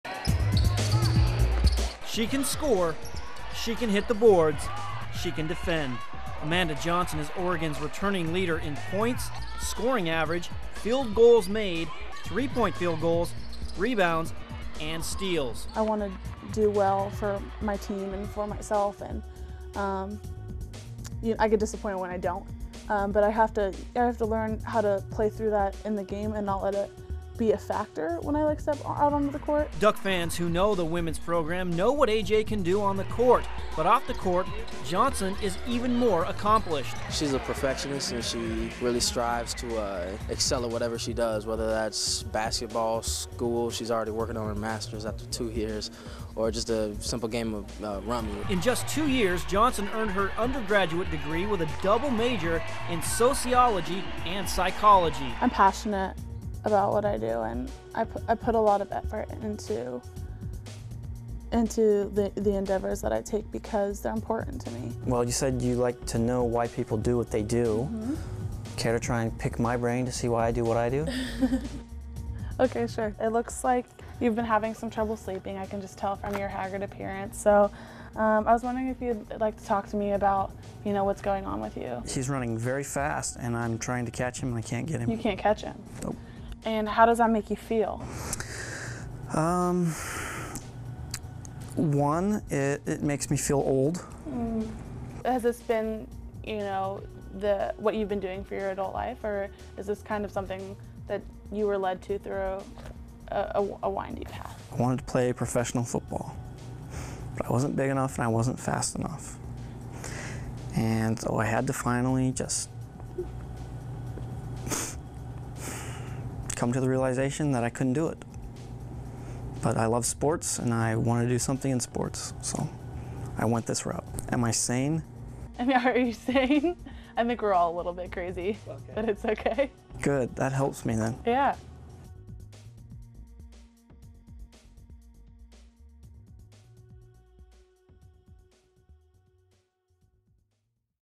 Amanda Johnson: Ballin' with Brains
Just a junior, Oregon's Amanda Johnson has already earned her undergraduate degree.  She's currently pursuing her master's in couples and family therapy. As Dirk Weishaar reports, AJ not only has the brains, but the athletic ability to succeed in Paul Westhead's up-tempo system.